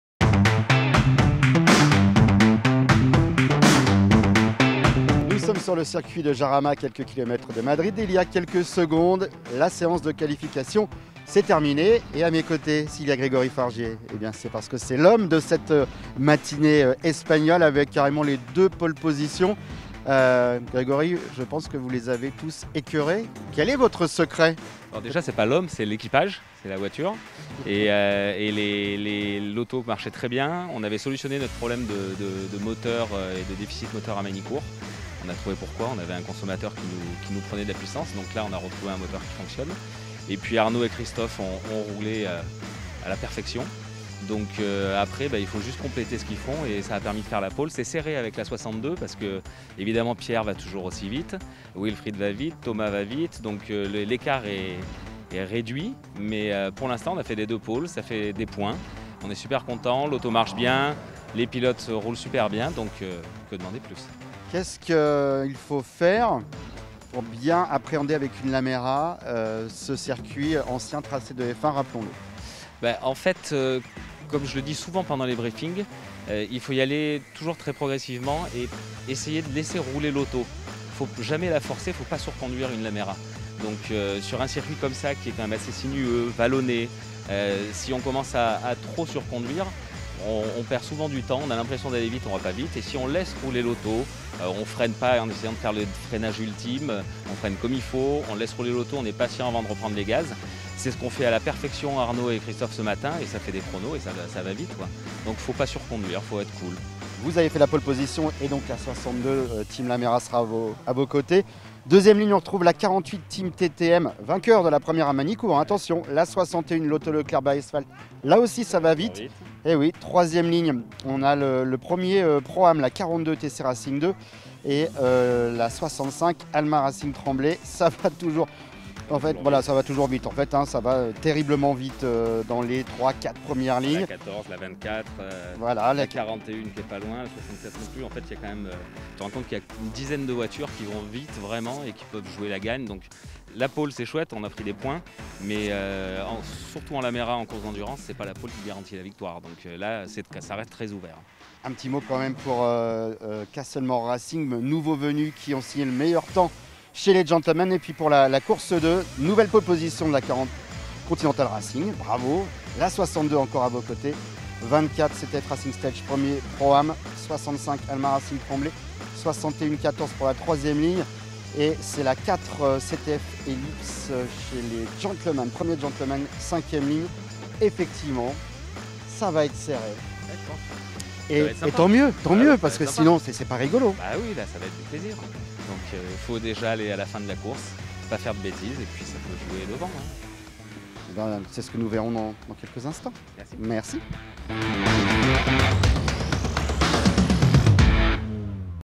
LAMERA CUP 2021 I Qualifications Course 2 du 22&23 mai à Jarama
Retour en images sur la séance de qualification de ce week end sur le Circuito del Jarama - RACE et les 2 pôles signées par Continental Racing.